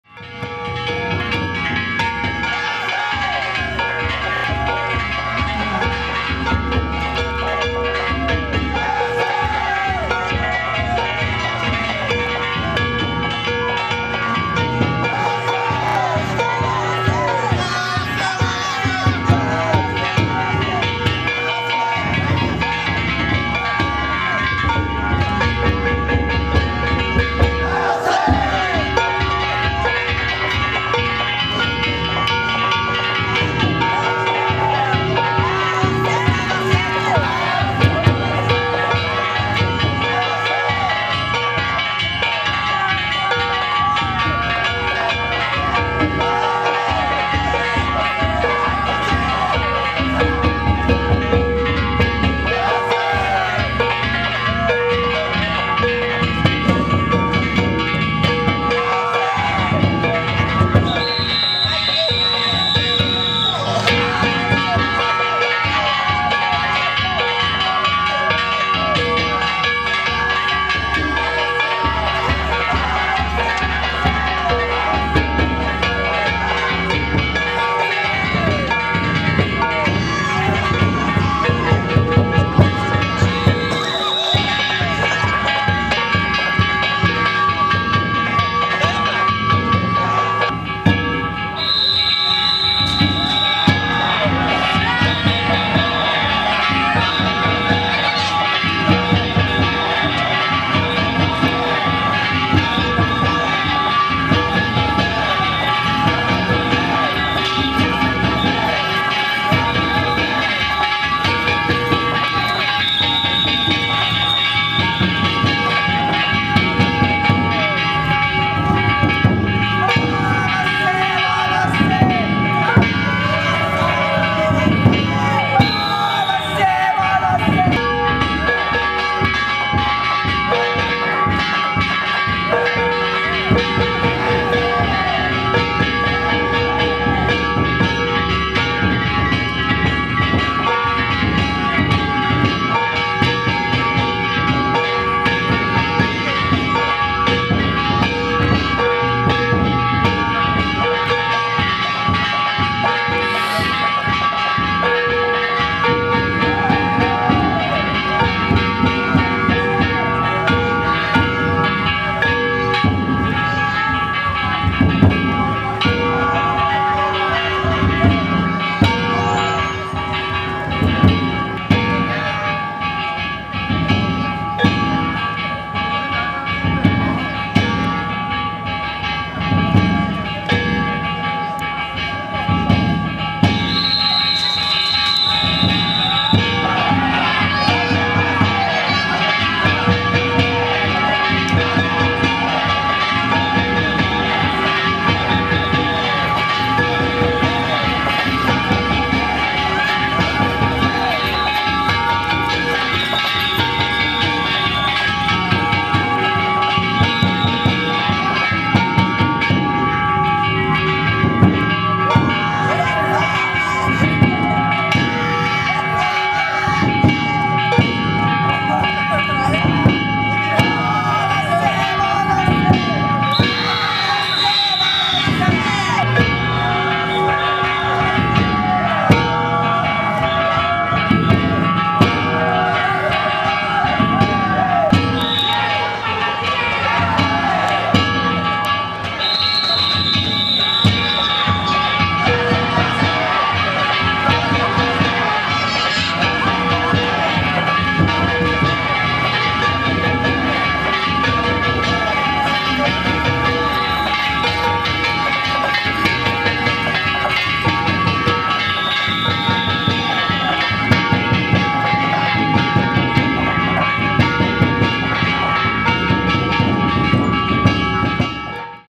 2009年 東灘区 青木區 だんじり 練り回し・トバセ
2009年
神戸市東灘区 青木 だんじり
夕方近く、駅前の練り回しと・公園付近での トバセ・戻せ